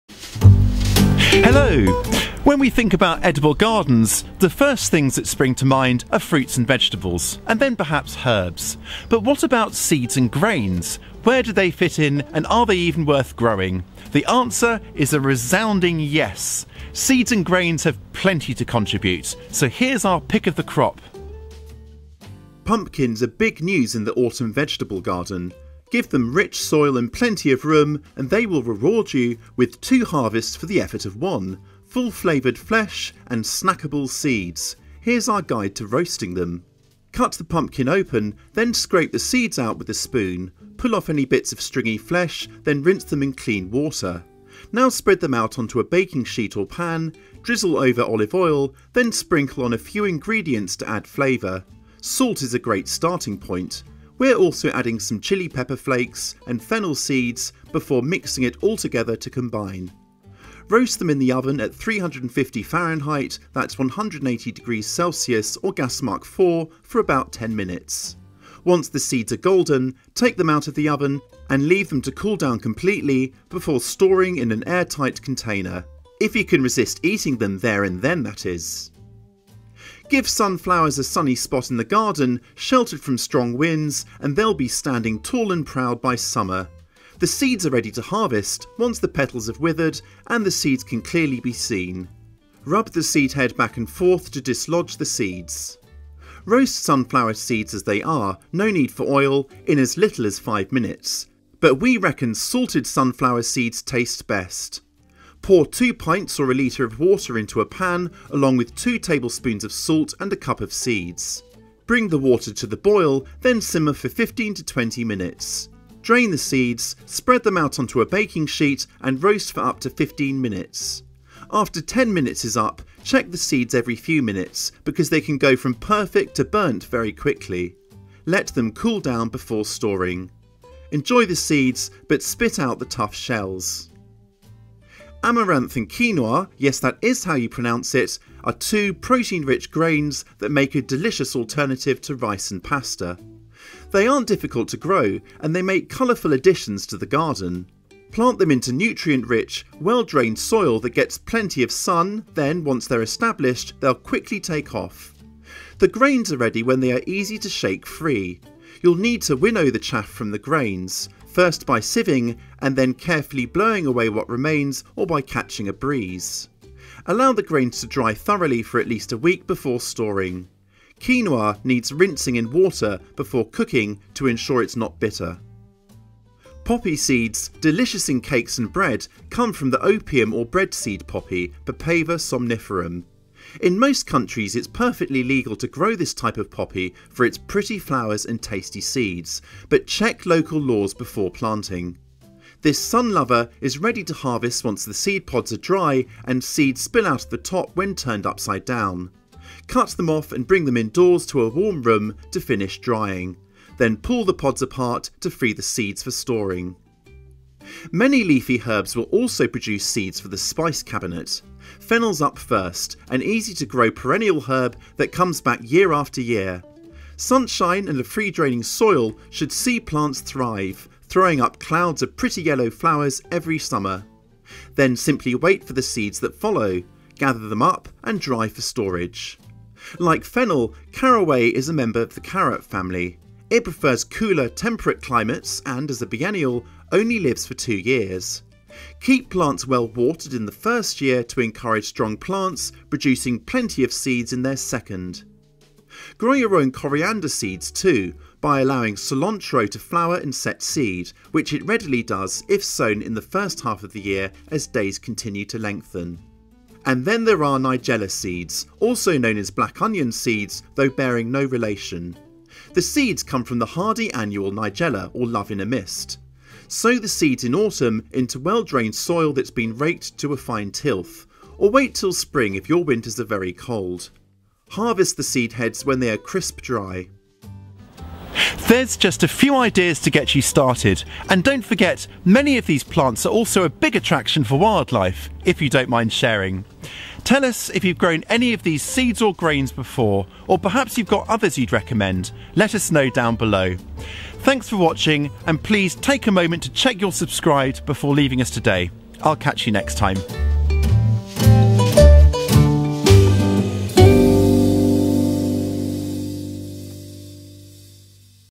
Easy to Grow Edible Seeds and Grains
Edible gardens aren’t just about fruits and vegetables. There are plenty of delicious seeds and grains you can grow too.

Pumpkin seeds, sunflower seeds, quinoa and amaranth can all be grown in your garden, as well as seeds from herbs such as fennel. You can even use some flower seeds for baking.

In this short video we’ll share some of the easiest-to-grow seeds and grains with you. They’ll help fill the store cupboard and look amazing in the garden too.